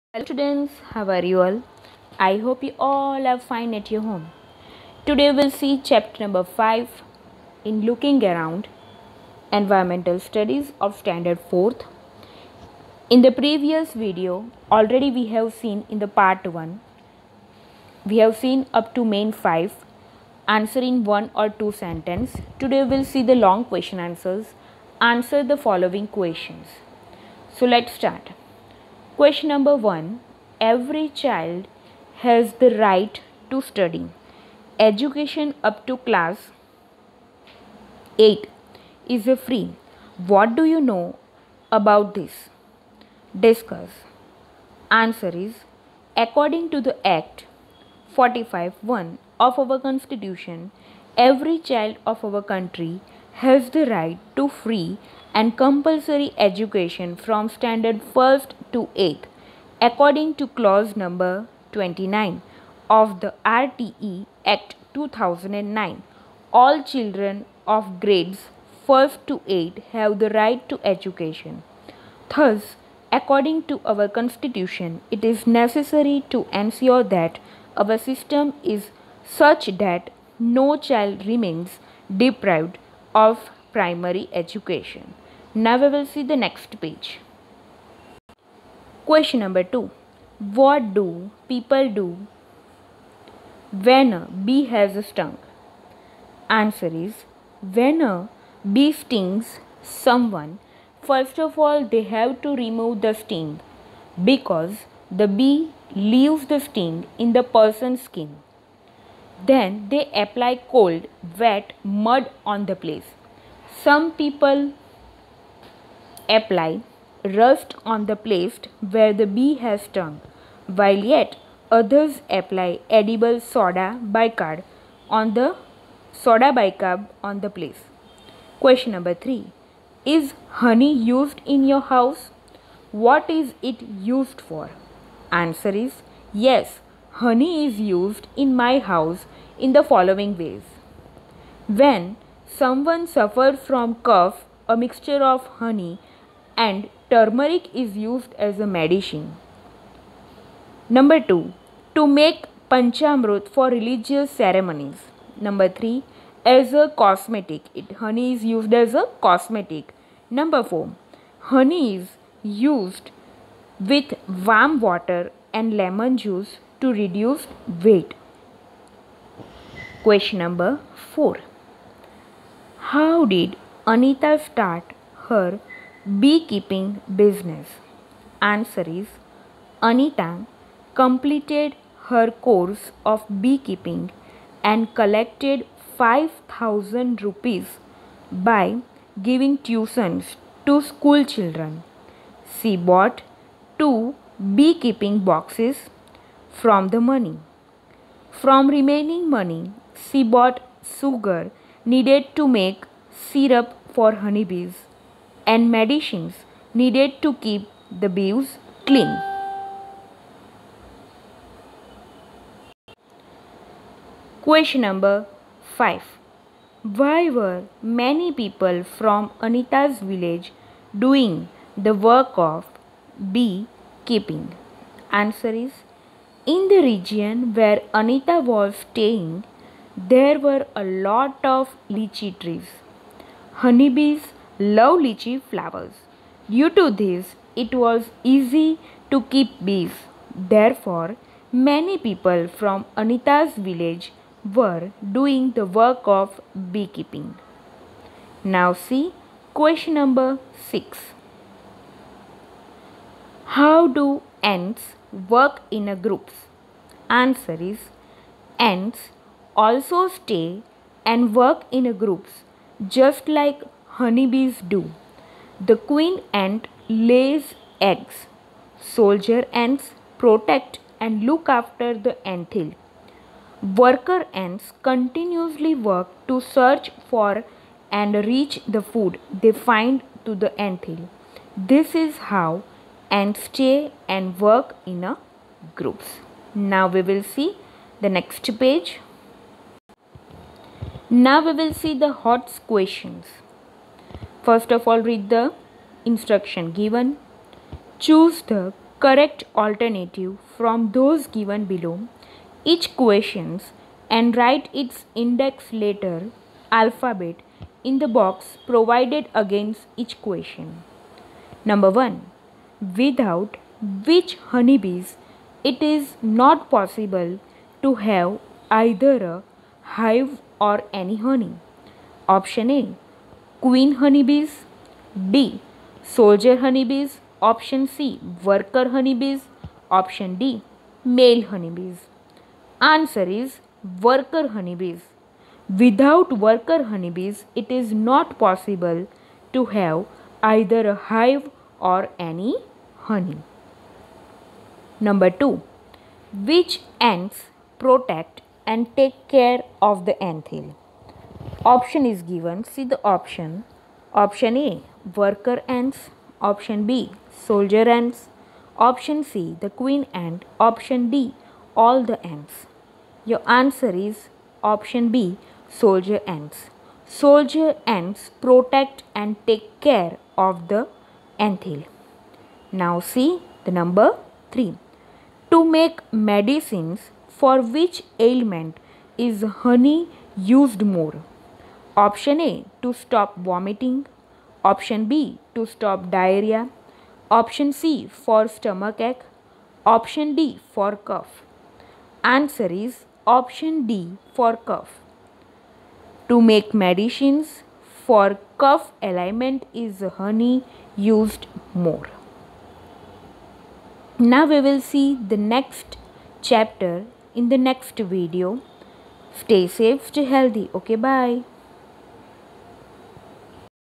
4th Std || Looking Around Environmental Studies Chapter 5 Ant And The Honeybees Workbook Part 2
4th Std Looking Around Environmental Studies Chapter 5 Ant And The Honeybees Workbook  Part 2